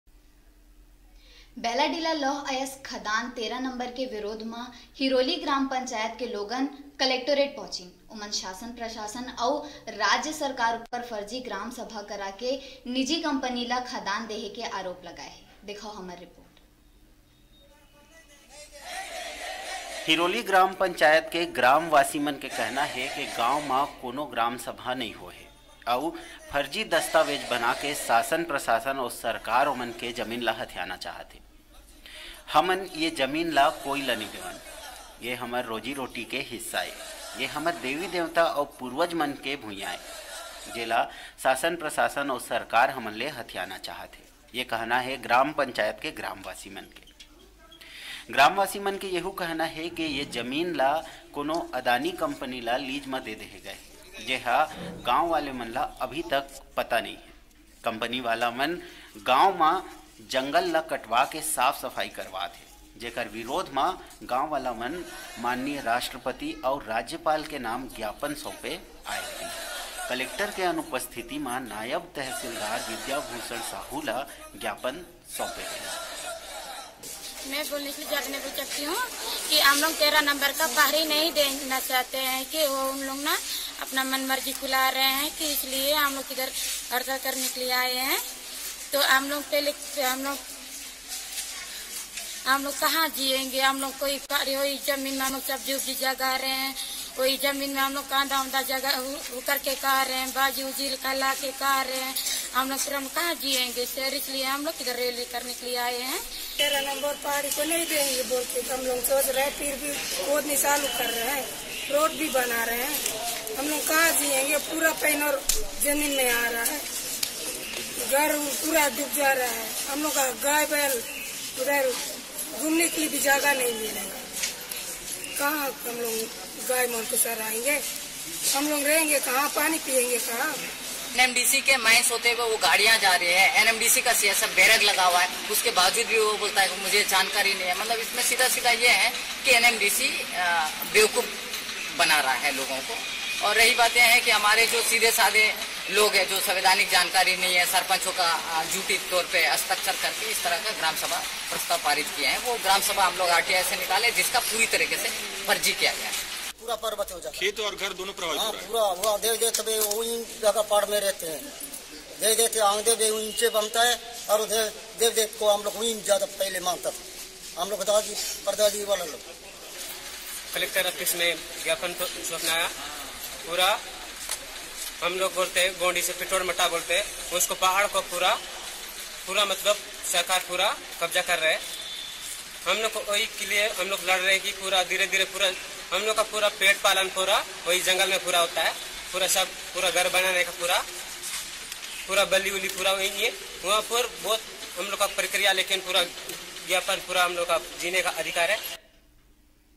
फ़र्ज़ी ग्राम सभा करा के निजी कंपनी ल खदान देहे के आरोप
दंतेवाड़ा ले डिजीटल रिपोर्टर मुकेश श्रिवास के रिपोर्ट...